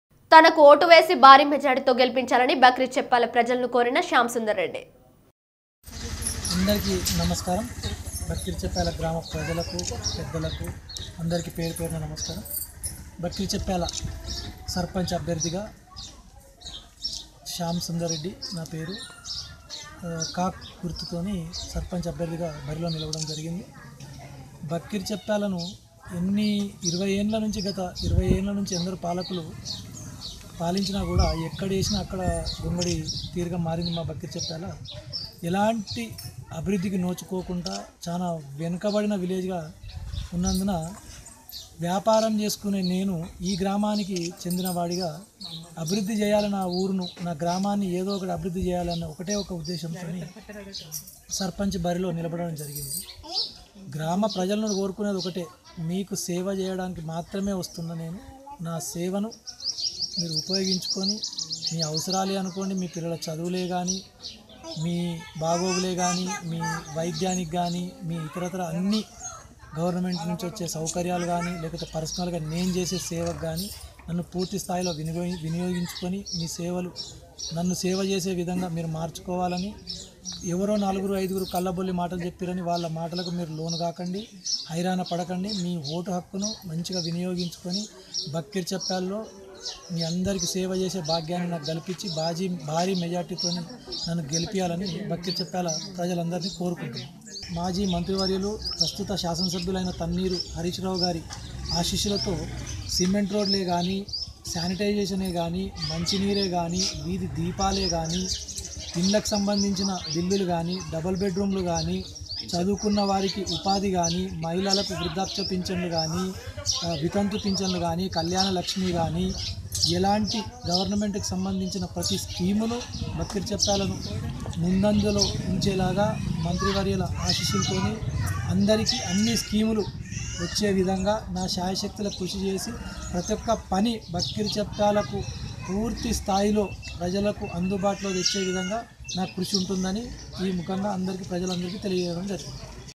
TS24 News బక్రీచెప్యాల్ లో ఎన్నికల ప్రచారం చేసిన శ్యామ్ సుందర్ రెడ్డి
లో ఎన్నికల ప్రచారం చేసిన శ్యామ్ సుందర్ రెడ్డి